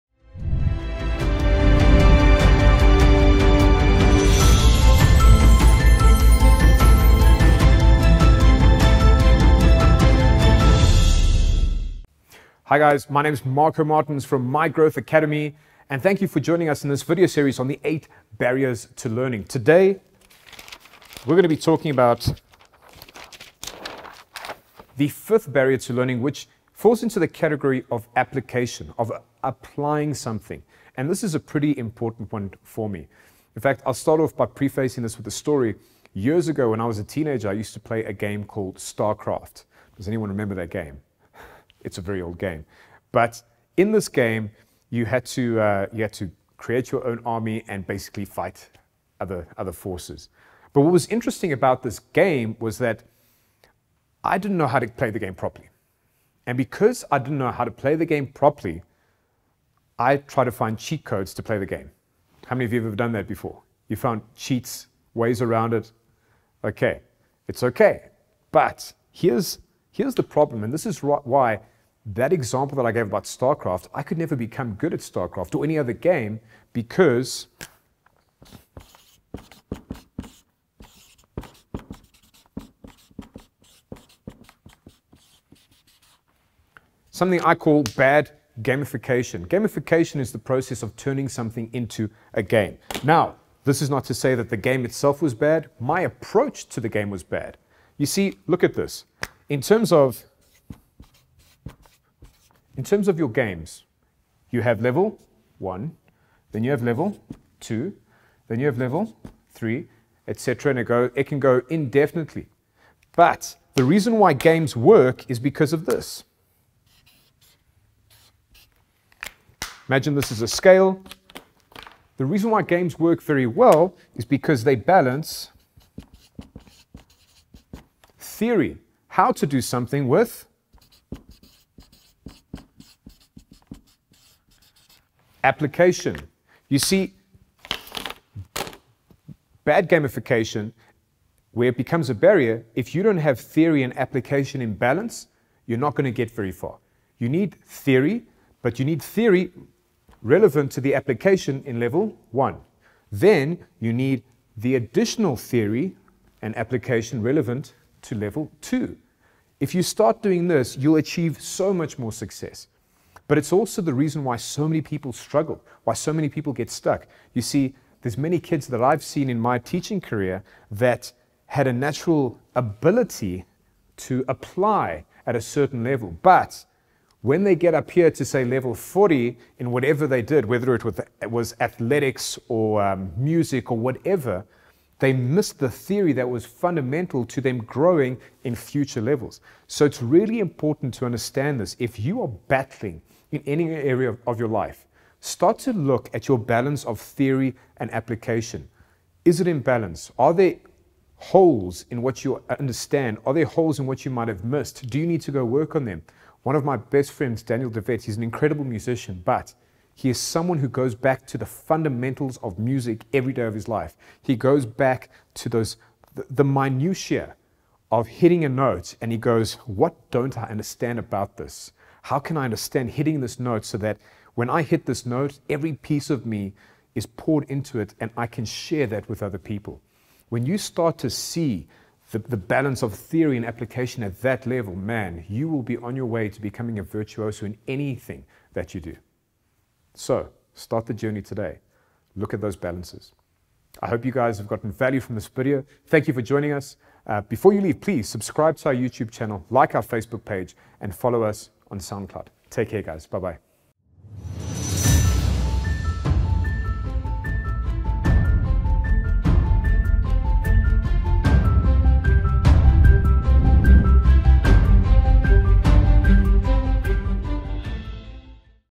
Application as a Barrier to Learning
To proceed to the next level, theory and application needs to be in balance.

What challenges do you face when it comes to application in learning?